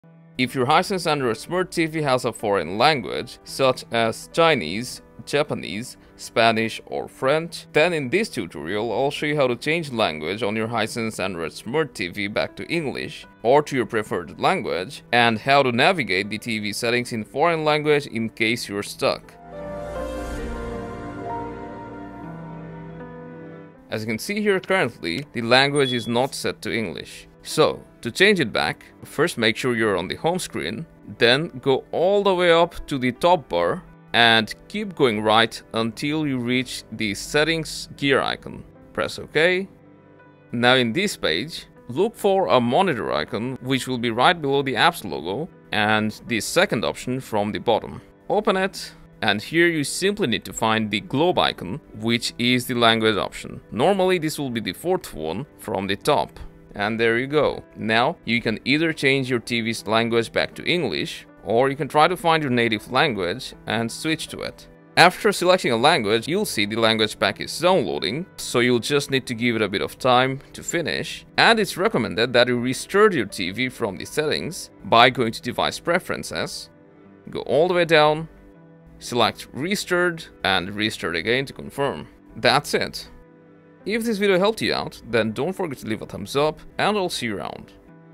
Language Change on Hisense Android Smart TV [Back to English]
Struggling to change your Hisense Smart Android TV Language Back to English when it's accidentally stuck in a foreign Language, like Japanese, Chinese, Spanish, French, and more? Need help to navigate HiSense Android TV when the default language is something you don’t know and want to switch back to English? No worries, we've got your Back

Simply follow the step-by-step beginner’s guide to change and set English as the default Language for Hisense Android TV.

0:00 Intro: change language on Hisense Android TV,
0:24 Change Language on Hisense TV, including Navigation when you don’t understand
1:12 Please Note